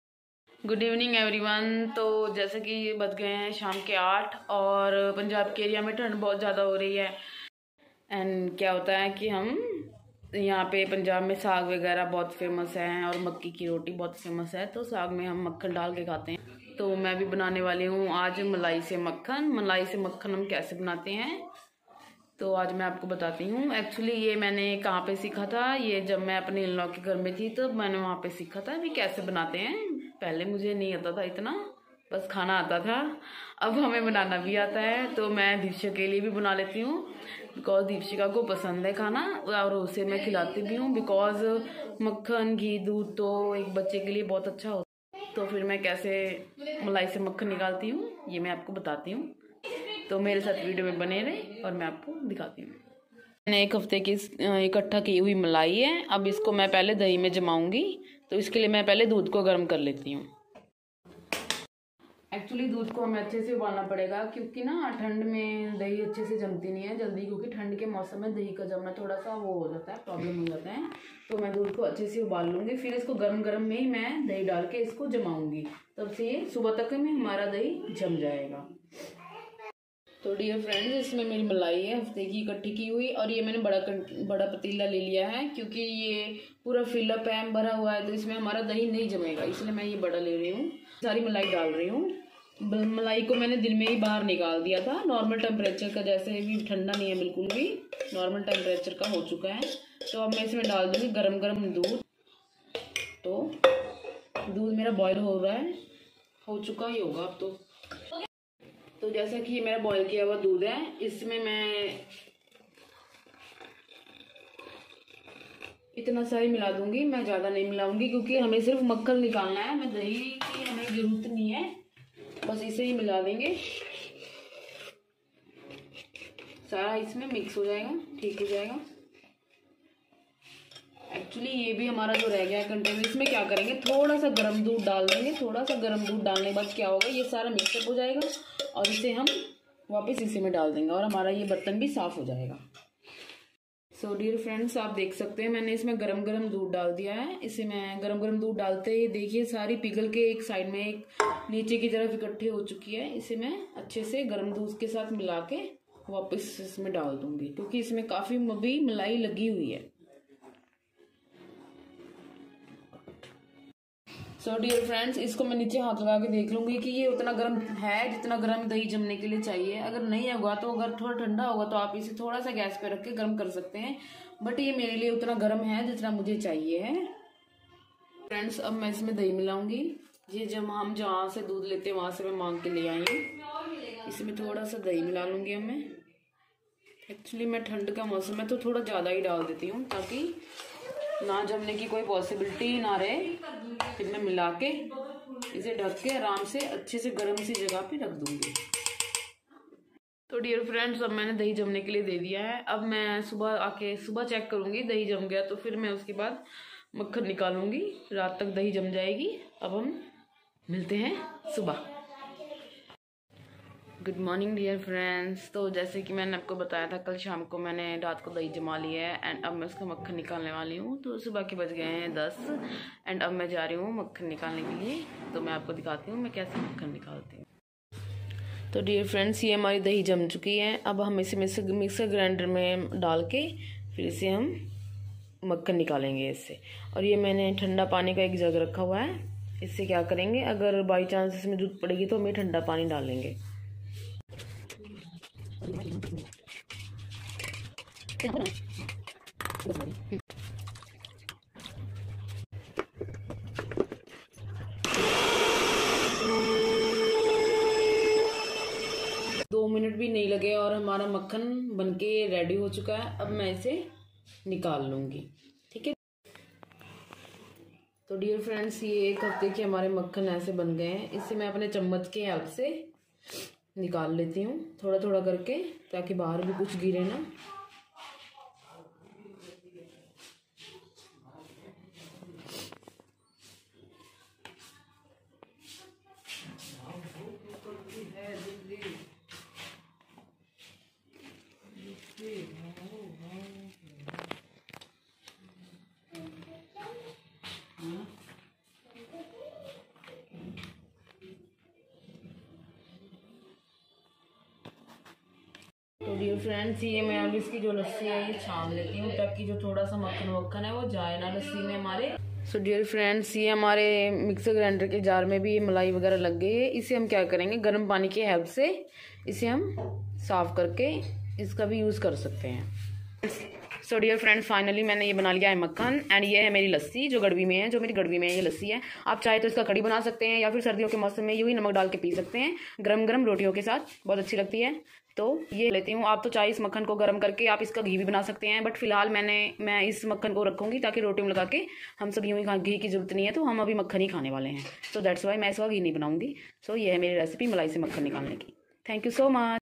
Malai se makhan sirf 2 mint me/desi style/Deepshikha and Suprita Vlogs
Hlo everyone i hope app sab es thand ke mosem me pure healthy honge .
es vlog me mein kaise makhan banati hu uski details ha
bhut asan kam hai ye to koi b kar sakte ha
me yahi request krti hu ek bar ere style se makhan jaroor bnaye kyuki ye desi style hai .